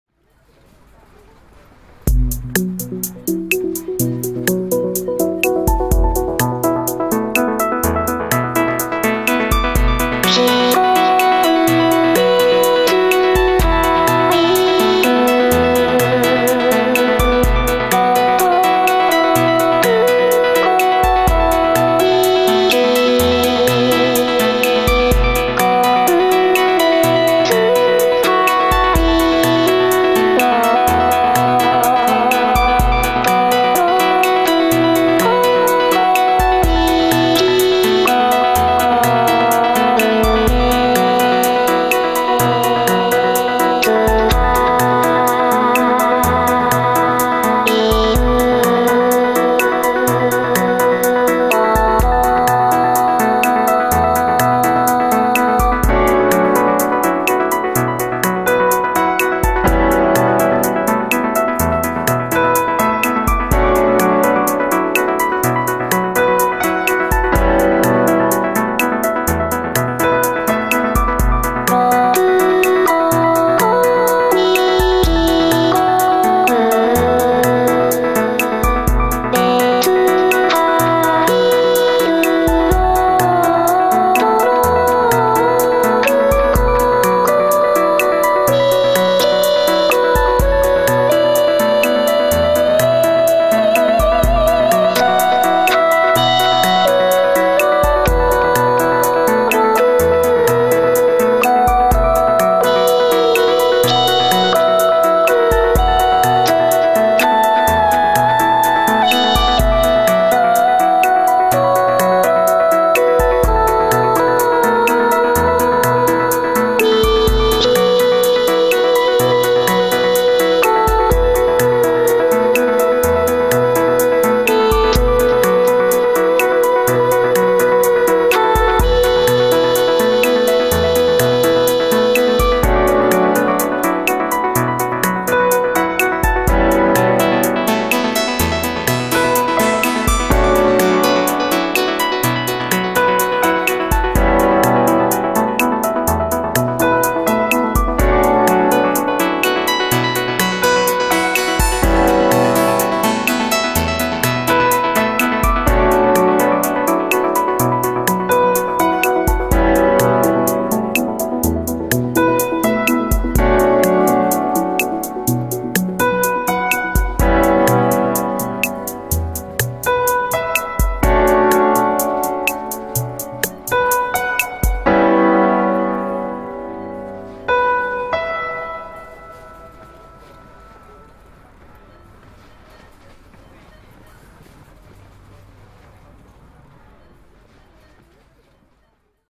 AquesTone Vocal Synthesizer
AquesTone is a vocal synthesizer plug-in (it is free!) It is a curious experience to sing a song with a keyboard... Try it yourself.